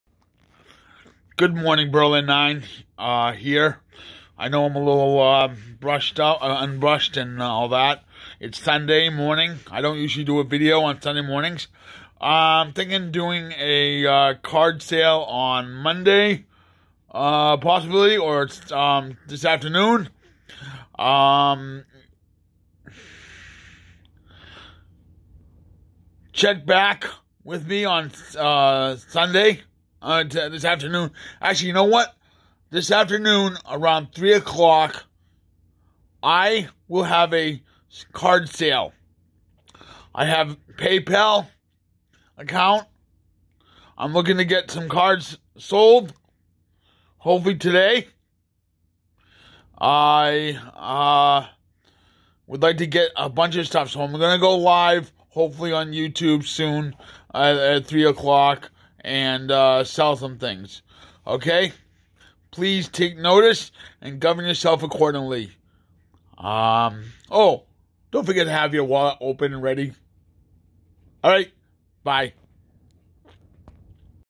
Sports card shop sale, many items.
Many sports card I'm Going to try to sell so I can get the money to buy other cards possibly.